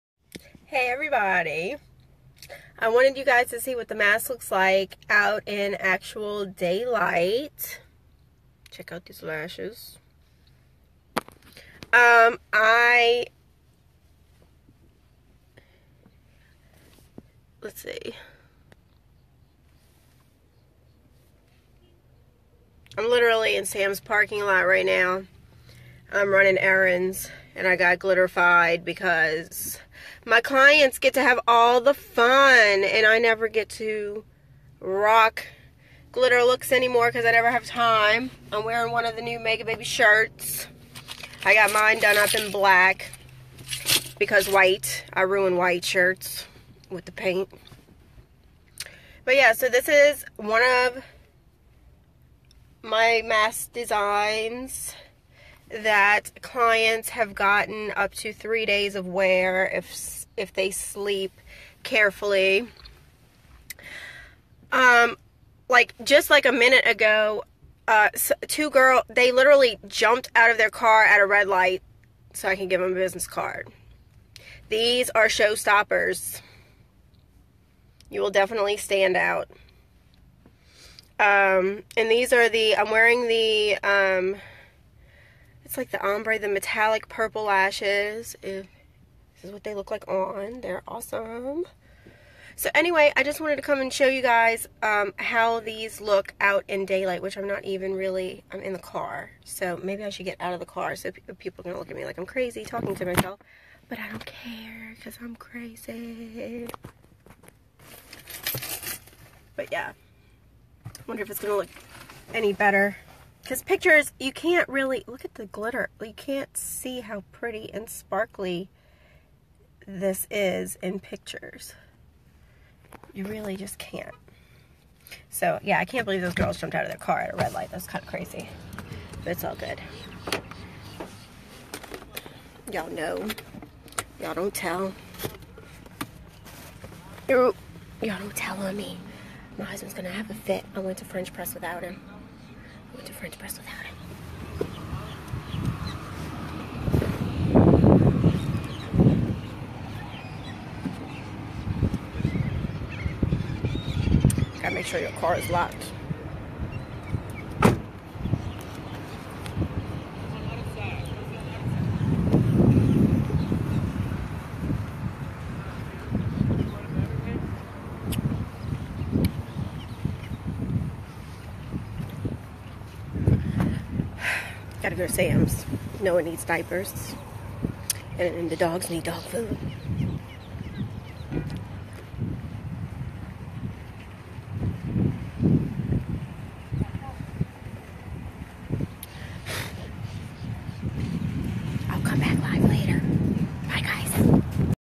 Mardi Gras mask makeup by artist Meagan Martinez of Meagababy Makeup
Short clip to show Meagababy Makeup Artist Meagan Martinez Mardi Gras Masks